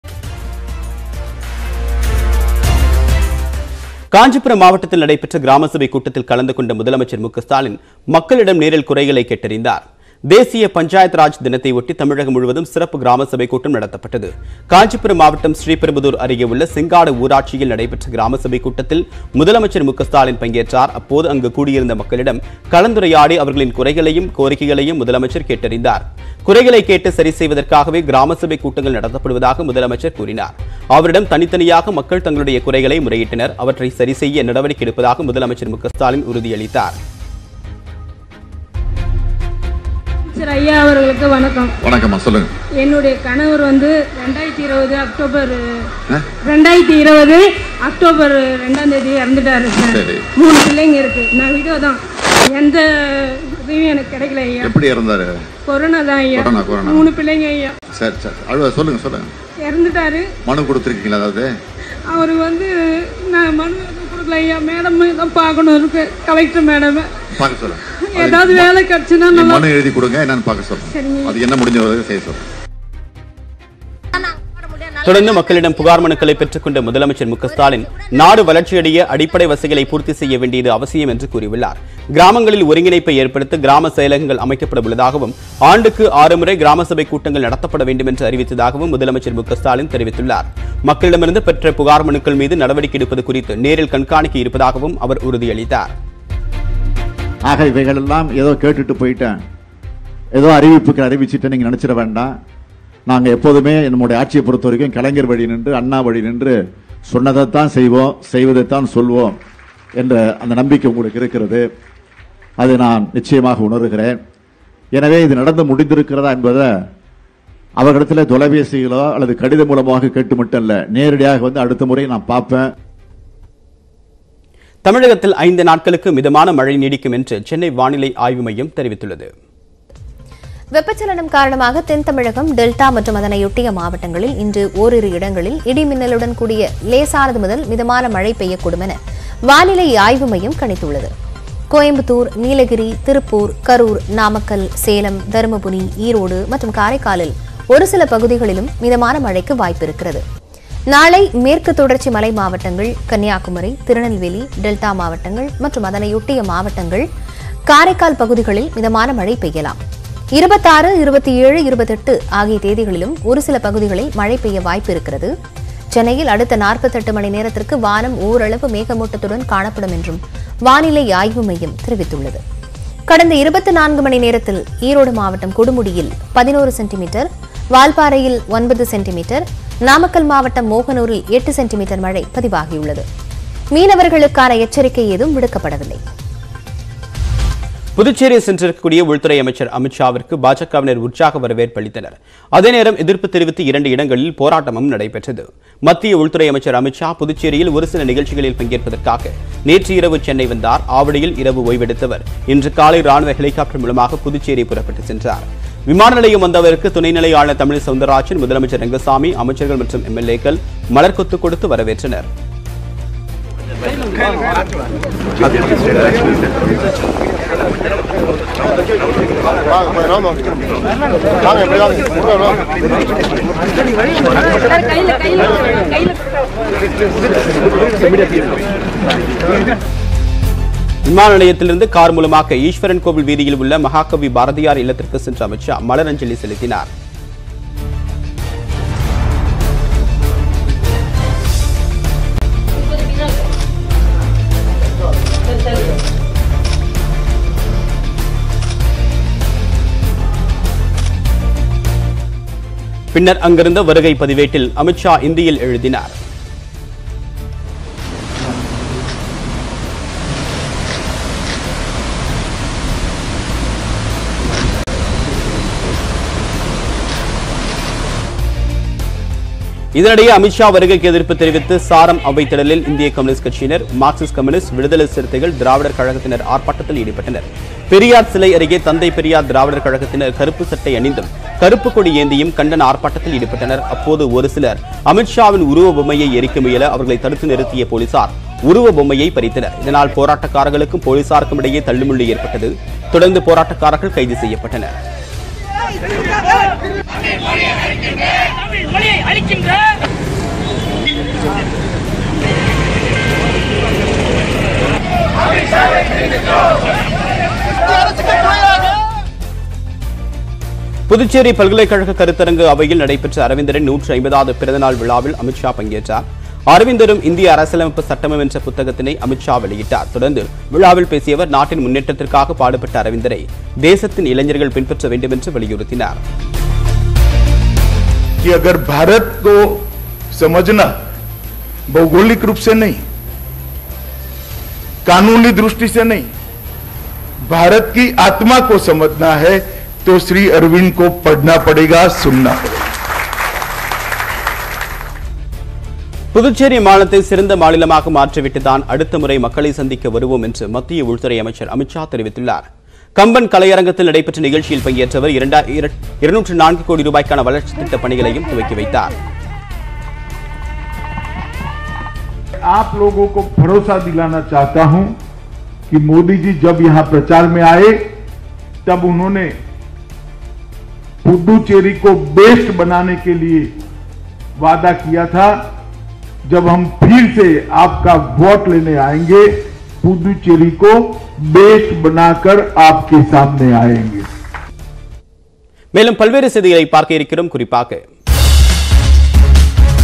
MK Stalin | Grama Sabha : செங்காடு கிராம சபை கூட்டத்தில் குறைகளையும், கோரிக்கைகளையும் கேட்டறிந்தார்
News18 Tamil Nadu | MK Stalin | TN Grama Sabha : செங்காடு கிராம சபை கூட்டத்தில்  குறைகளையும், கோரிக்கைகளையும் கேட்டறிந்தார் | செய்திகளை  துல்லியமாகவும் துரிதமாகவும் வழங்கும் தமிழகத்தின் முதன்மை செய்தி தொலைக்காட்சி உங்கள் நியூஸ் 18 தமிழ்நாடு.